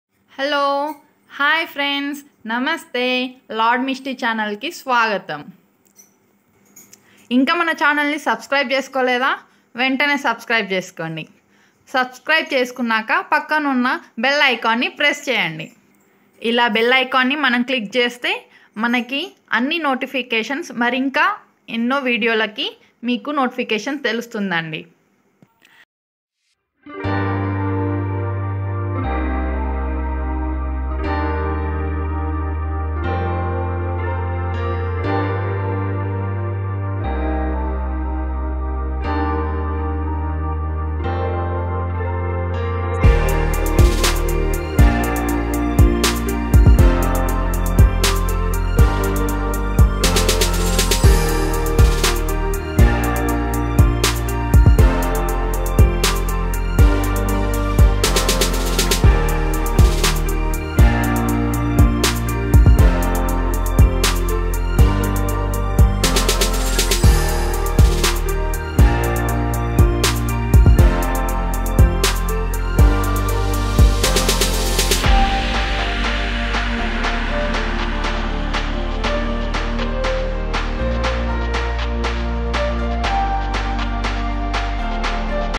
Best Hands Free Umbrella To Save From Rain and Sun| Best Online Purchase
Rutba Hands Free Umbrella Hat Sun & Rain for School Going Kids and Adults Umbrella  (Multicolor)

link to buy